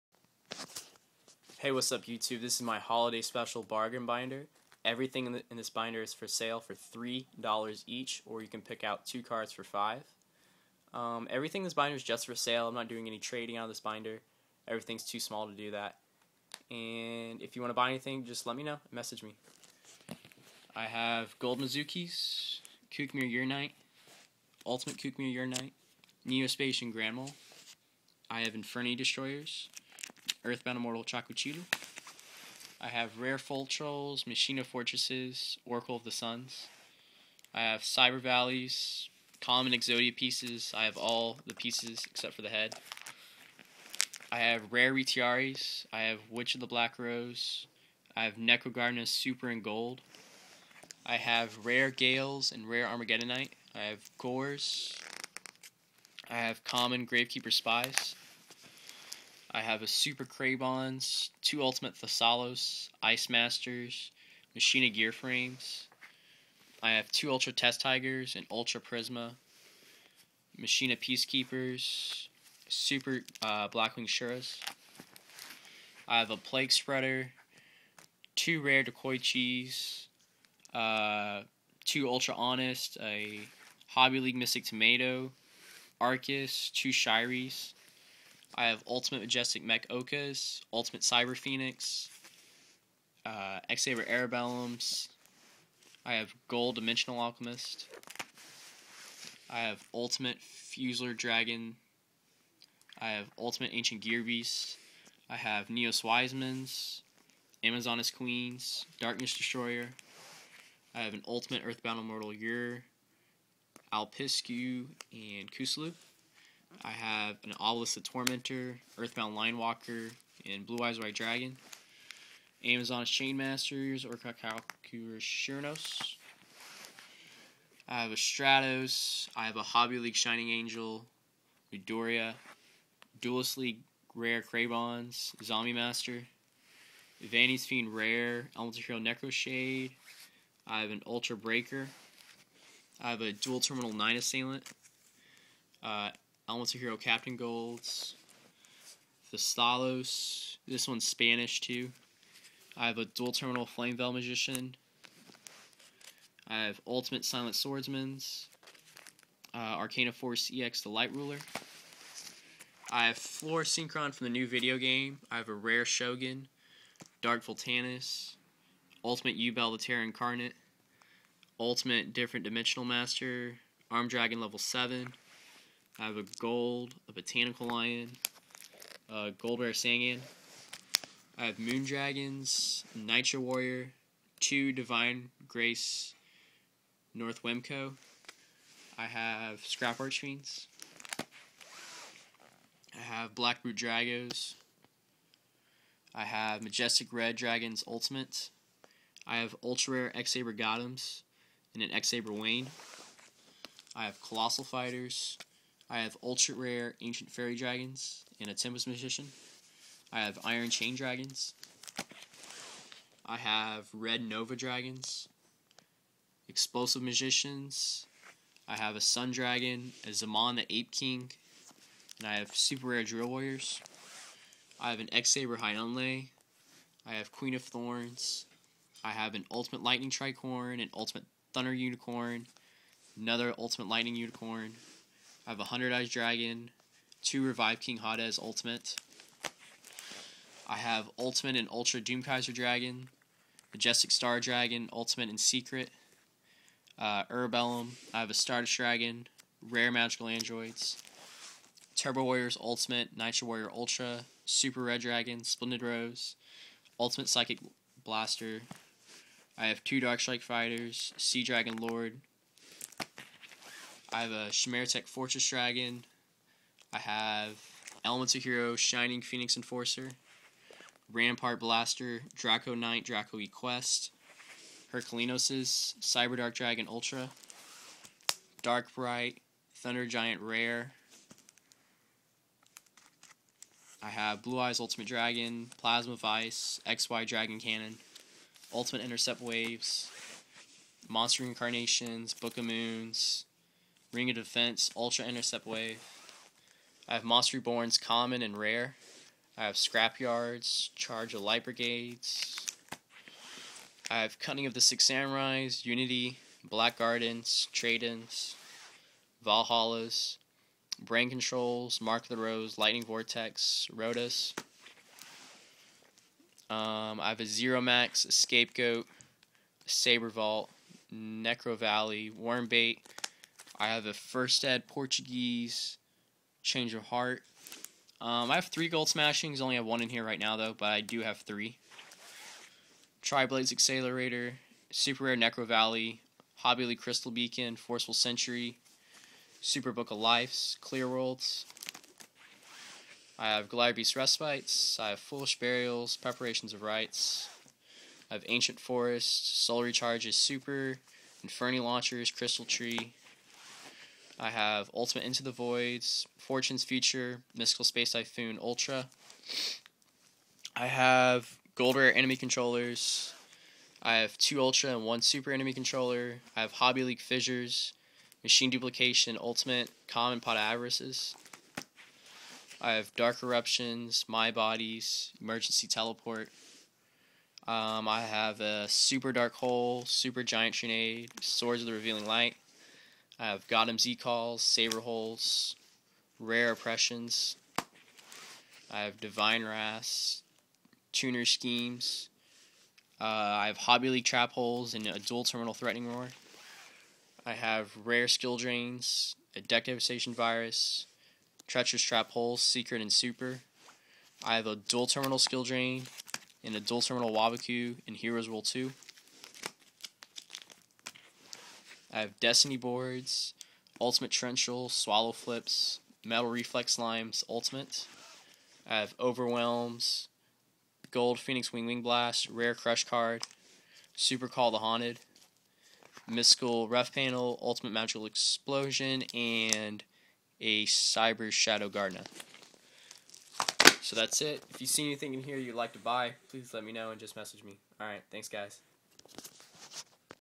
Kyle Bowling's Holiday Bargain Binder!!!
Hey cause I'm back and I got a bargain binder for you!!!
This binder is like my last one everything in here is for sell for a low price of just $3!!! And if you wanna get two cards I'll give you a discount and make it $5!!!
**Cards are ONLY FOR SELL NOT TRADE
**If you have any questions just read my rules and regs of trading
Everything must go so tell your friends!!!